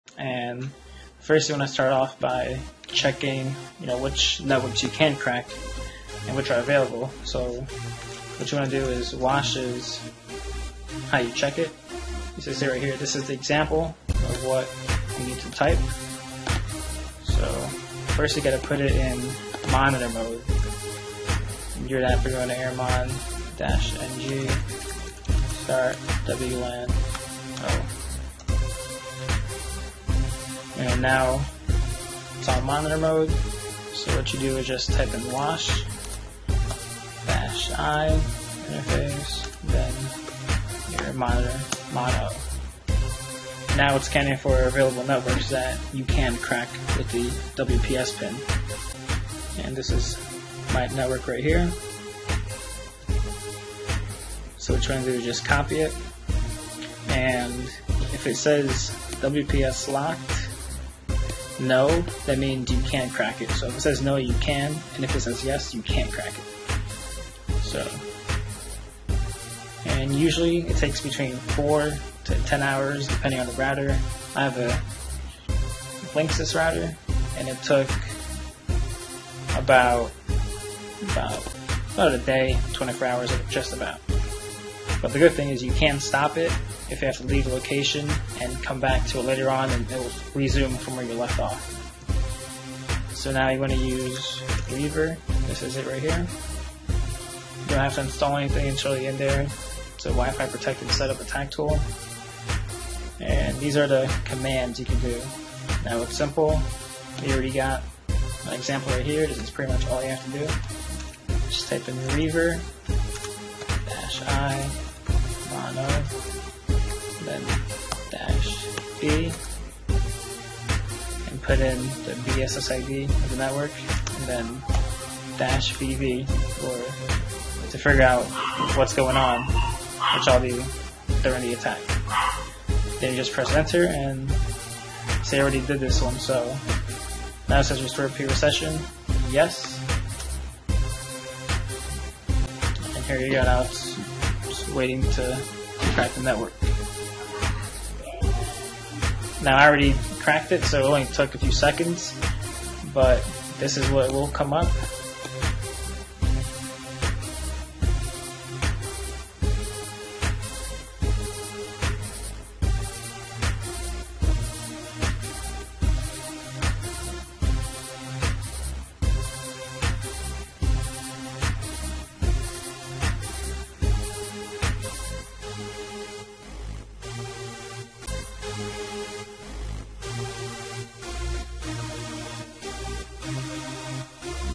How To Crack WPA-WPA2 WPS Using Reaver & Backtrack 5r3 (NO DICTIONARY)
I show you how to crack wpa-wpa2 by getting the wps pin using reaver with backtrack.